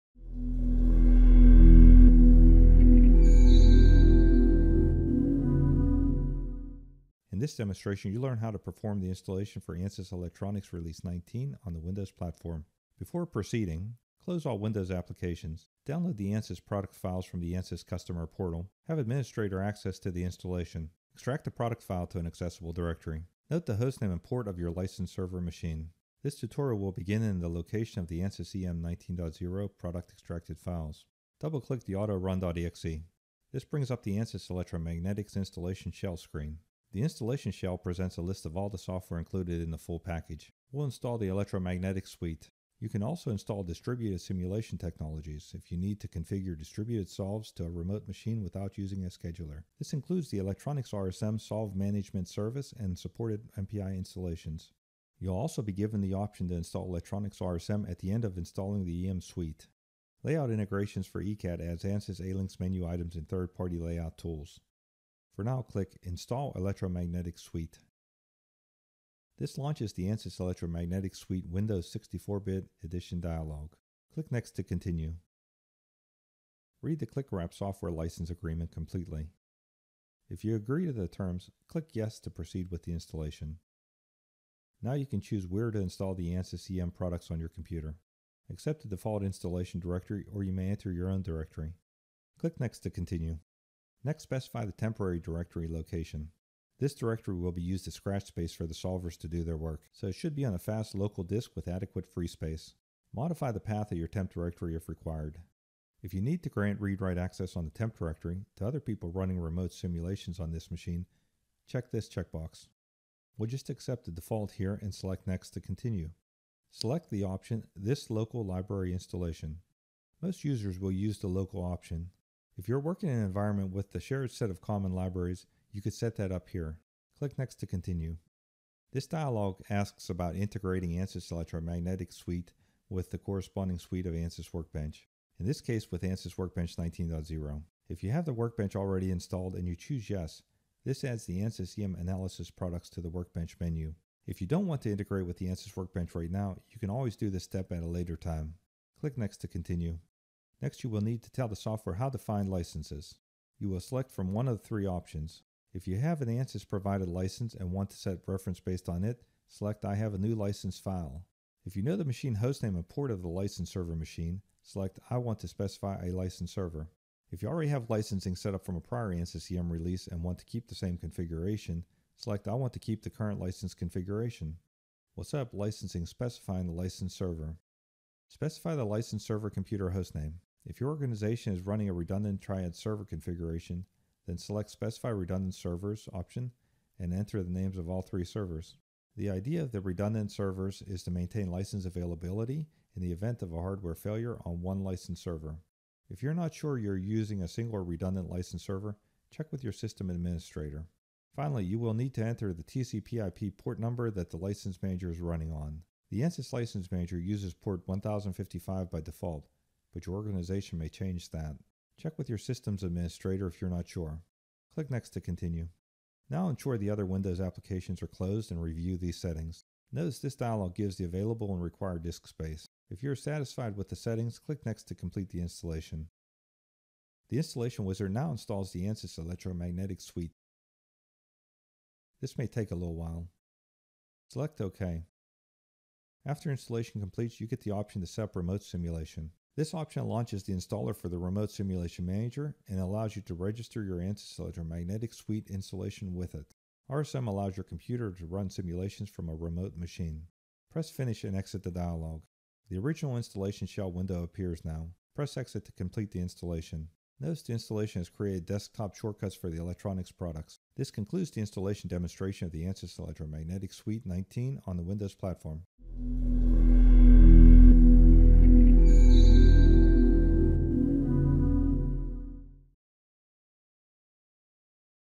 Installing the ANSYS Electromagnetics Suite on Windows
This video demonstrates how to install the ANSYS, Inc. Electronics Suite on the Windows Platform.